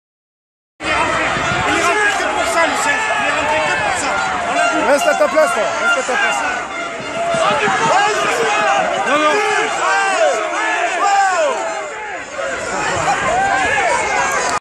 BAGARRE GÉNÉRALE ENTRE EGLETONS ET CHASTEAUX
Bagarre générale entre égletons et chasteaux pendant les finales territoriales du limousin sur le terrain annexe du stadium de Brive qui s'est soldée par un carton rouge des deux côtés.
(les joueurs sont allés boire une bière a la fin du match)